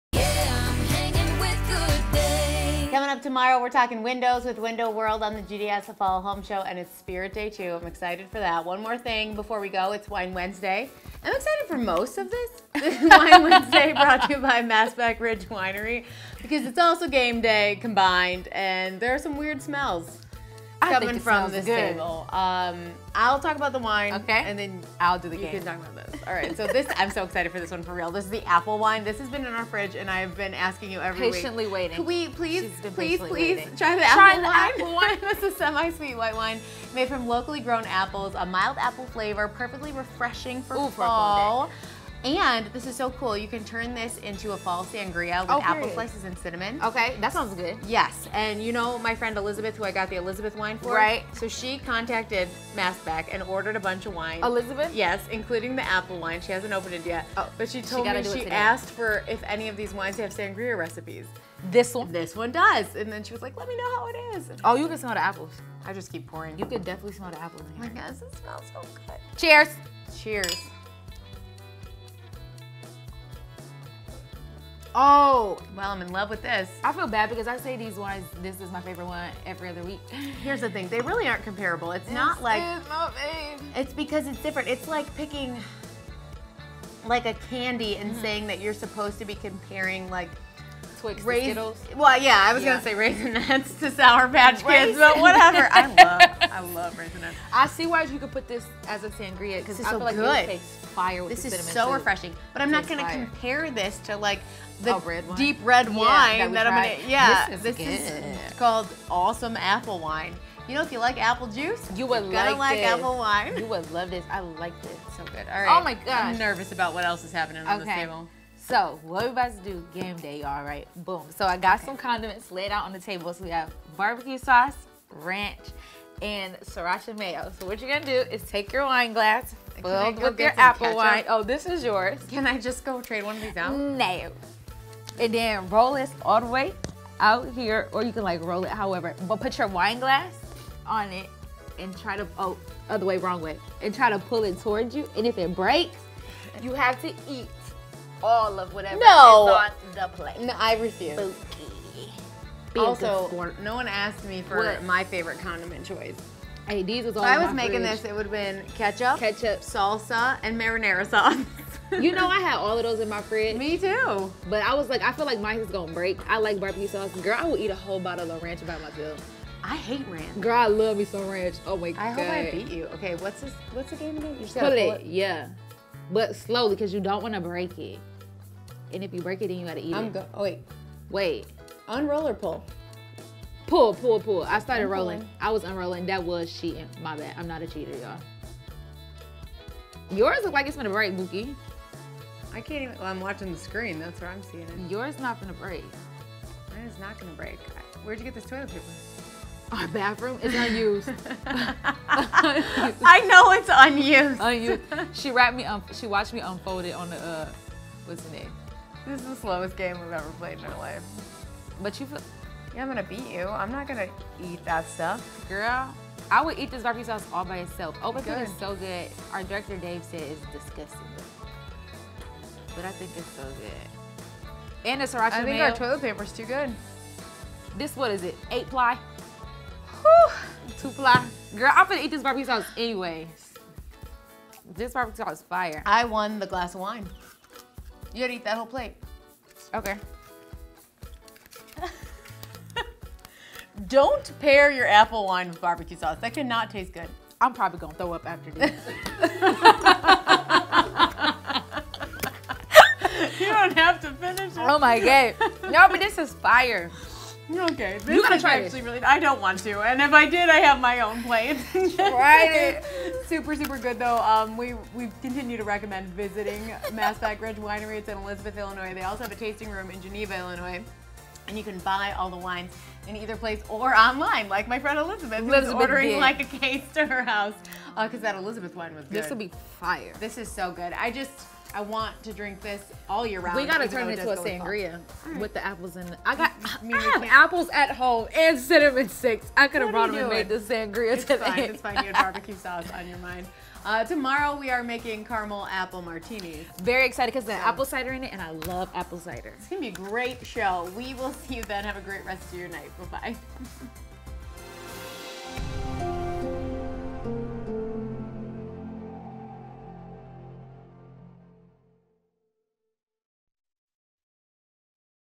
Transitioning from Summer to Fall Wine with Massbach Ridge Winery’s Apple Wine
Happy Wine Wednesday! Michelle and Taminique are celebrating Fall early with Massbach Ridge Winery’s Apple Wine. Their Apple Wine is a semi-sweet white wine made from apples grown locally. If you’re looking to spice things up a bit, you can turn this wine into a Fall sangria with fresh apple slices and cinnamon sticks. You’re able to pick this wine up from the winery or their tasting room in Galena!